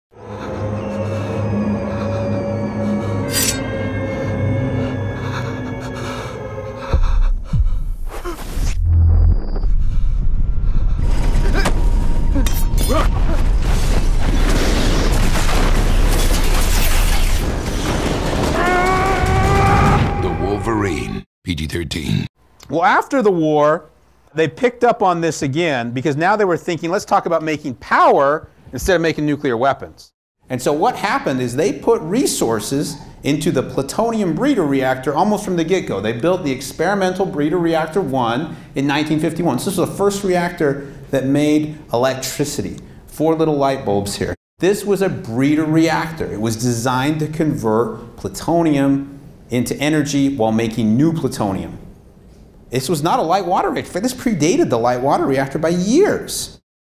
1h10m04s25f After War EBR1 Powered 4 Lightbulbs TR2016a
Well, after the war, they picked up on this again because now they were thinking- Let's talk about making power instead of making nuclear weapons.

And so what happened is they put resources into the plutonium-breeder reactor almost from the get-go.

They built the Experimental Breeder Reactor One in 1951.

This was the first reactor that made electricity.

Four little light bulbs here.
This was a breeder reactor.

It was designed to convert plutonium into energy while making new plutonium.

This was not a light water reactor!
This predated the light-water reactor by years!